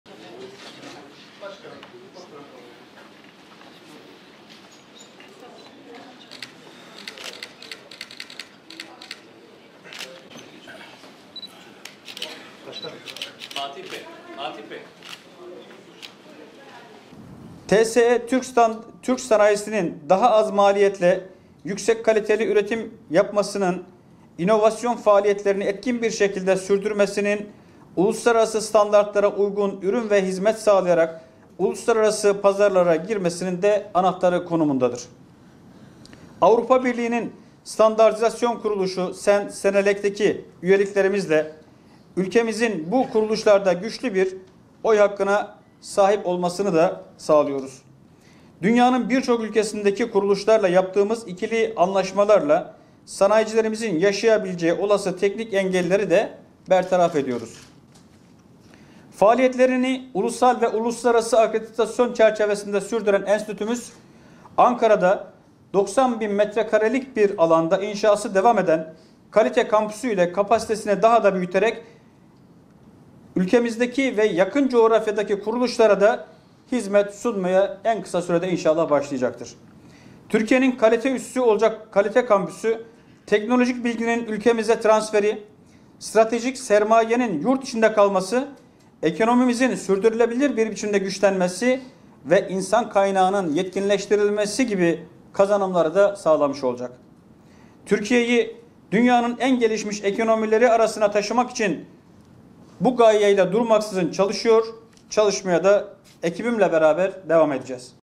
"TSE ile Sektör Buluşmaları" etkinliği yapıldı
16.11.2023
Türk Standartları Enstitüsü (TSE) Başkanı Mahmut Sami Şahin, "TSE, Türk sanayisinin daha az maliyetle yüksek kaliteli üretim yapmasının, inovasyon faaliyetlerini etkin bir şekilde sürdürmesinin, uluslararası standartlara uygun ürün ve hizmet sağlayarak uluslararası pazarlara girmesinin anahtarı konumundadır." dedi.